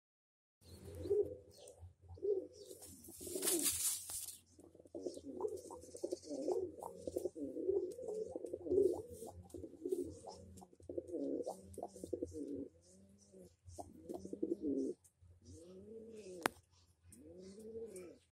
Kabutar for sale in rs700 //sale //kabutar//high flyer
Hi f dosto ye kabutar sale hai or ₹700 bechne ka hai agar apko chahiye to comment me apna number dale.